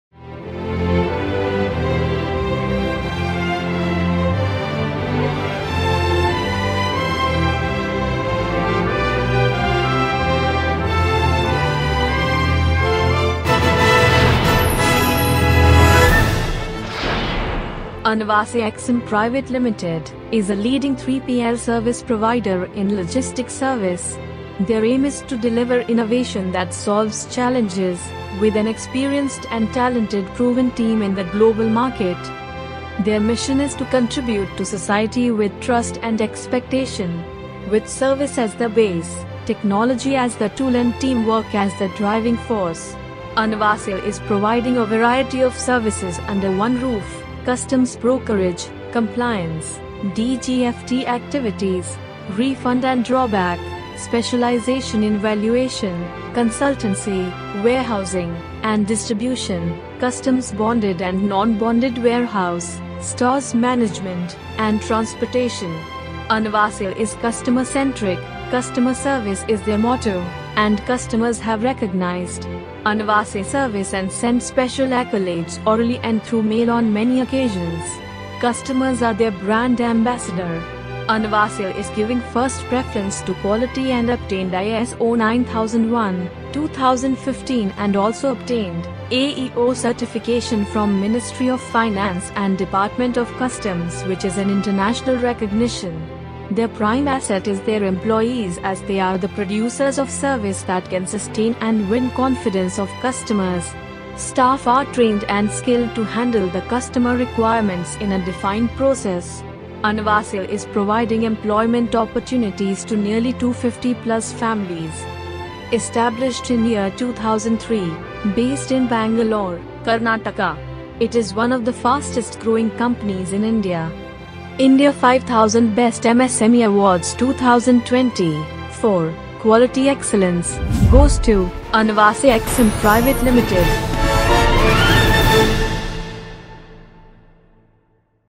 Anvase Exim Pvt Ltd - Winner of India 5000 Best MSME Awards 2019
Anvase Exim Pvt Ltd is a leading 3PL Service Provider in LOGISTICS SERVICE. Their aim is to deliver innovation that solves challenges, with an experienced and talented proven team in the global market.

Their mission is to contribute to society with trust and expectation. With service as the base, technology as the tool & team work as the driving force. Anvase is providing a variety of services under one roof, Customs Brokerage, Compliance, DGFT activities, Refund and Drawback, Specialization in Valuation, Consultancy, Warehousing and Distribution, Customs Bonded and Non Bonded Warehouse, Stores Management and Transportation. Anvase is customer centric, customer service is their motto, and Customers have recognized. Anvase service and sent special accolades orally and through mail on many occasions.  Customers are their Brand Ambassador. Anvase is giving first preference to quality and obtained ISO 9001 – 2015 and also obtained AEO Certification from Ministry of Finance & Department of Customs which is an international recognition. Their Prime Asset is their employees as they are the producers of service that can sustain and win confidence of Customers. Staff are trained and skilled to handle the customer requirements in a defined Process. Anvase is providing employment opportunities to nearly 250+ families.

Established in the year 2003. Based in BANGALORE, KARNATAKA It is one of the fastest-growing companies in India.

India 5000 Best MSME Awards 2020, For Quality Excellence, Goes to, ANVASE EXIM PVT LTD.